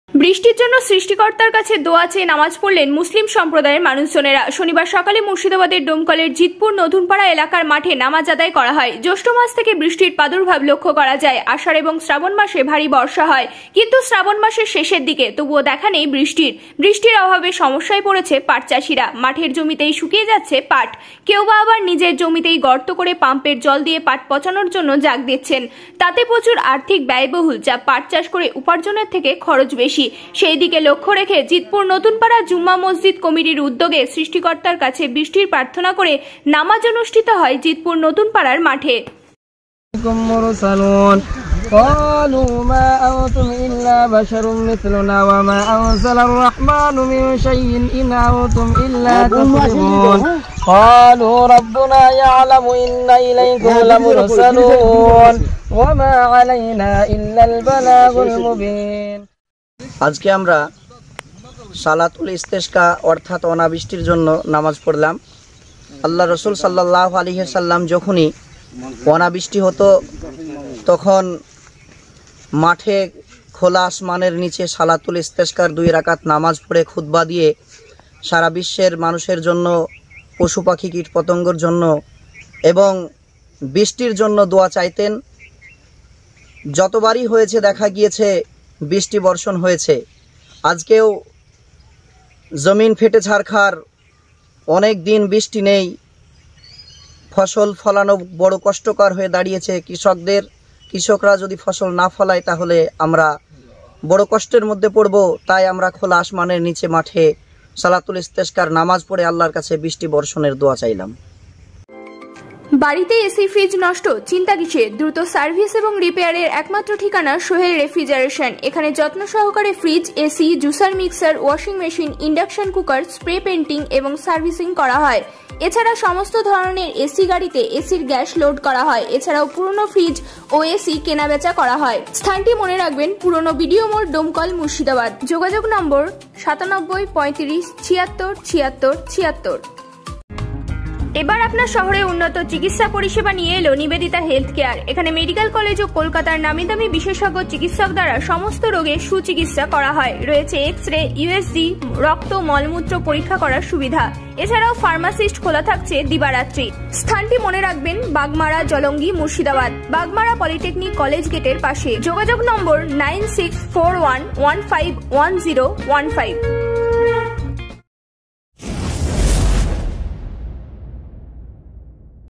অনাবৃষ্টির কারনে 'নামাজ' আদায়
অনাবৃষ্টির কারনে নামাজ আদায় করলেন মুসলিম সম্প্রদায়ের মানুষজনেরা